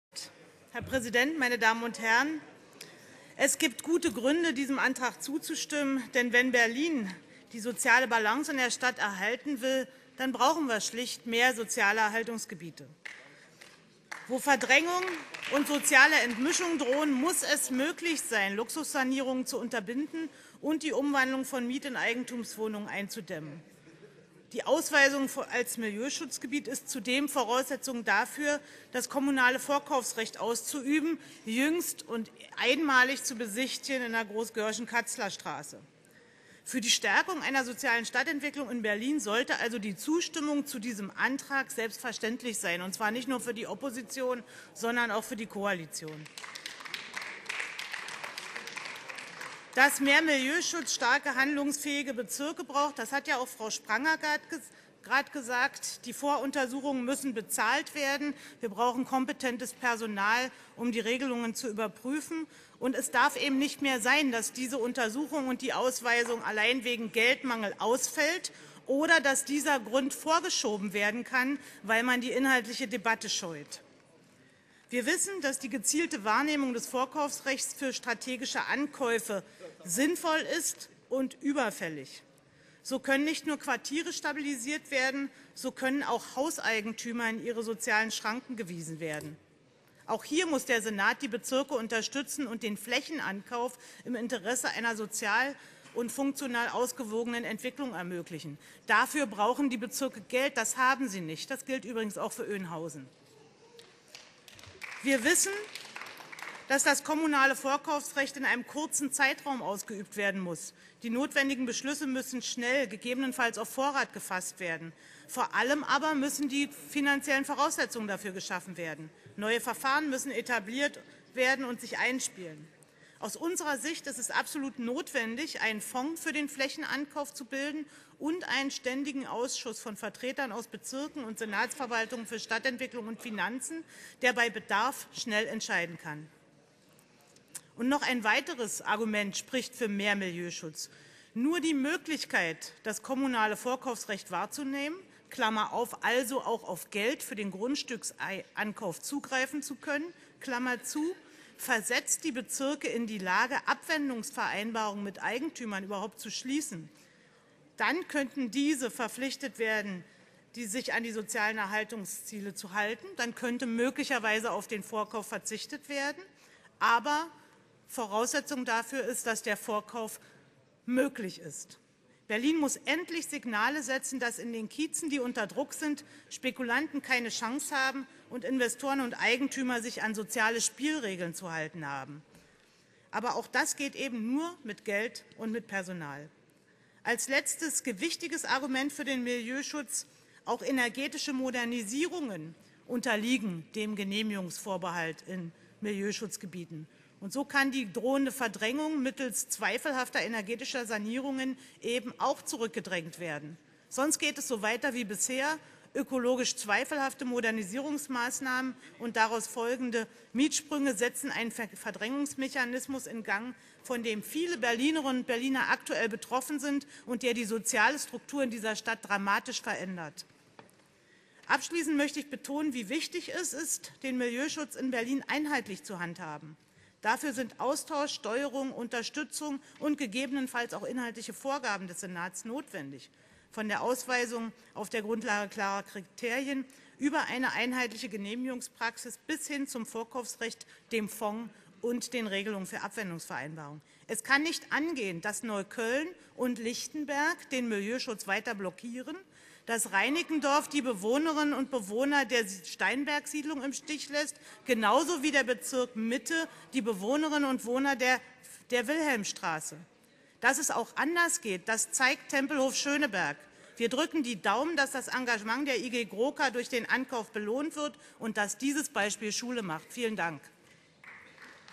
63. Sitzung - Katrin Lompscher zu Berlin braucht mehr “Soziale Erhaltungsgebiete”
24. April 2015
63. Sitzung des Berliner Abgeordnetenhauses

betont, Berlin müsse ein Signal setzen, dass sich Investoren und Spekulanten auf dem Berliner Wohnungsmarkt an soziale Spielregeln halten müssten. Dafür bedürfe es mehr sozialer Erhaltungsgebiete. Denn diese ermöglichten ein kommunales Vorkaufsrecht. Nur so könnten Quartiere stabilisiert und Hauseigentümer in die Schranken gewiesen werden.